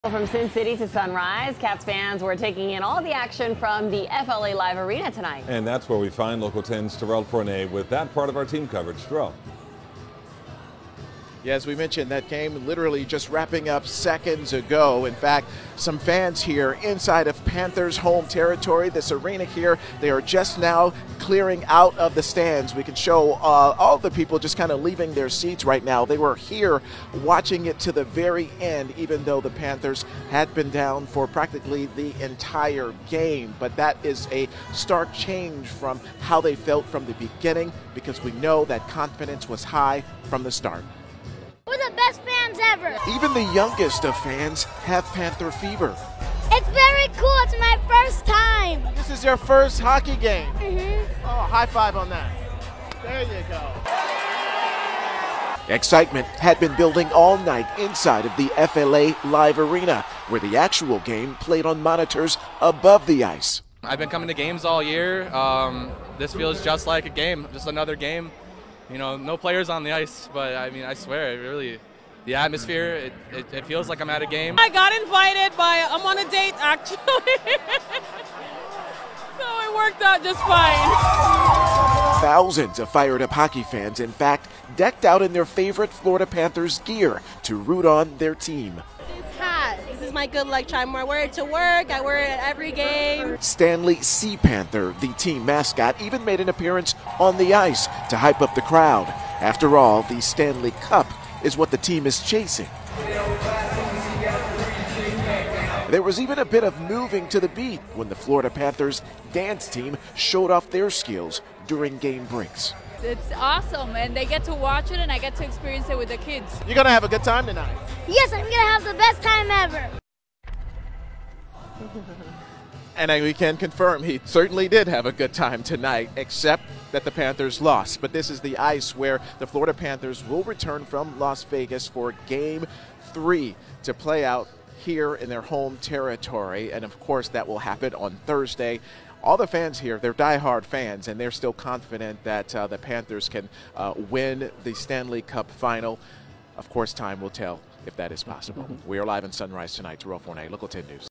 Panthers fans pack FLA Live Arena for Game 2 watch party
Panthers fans came together at FLA Live Arena on Monday night for a Stanley Cup Final Game 2 watch party.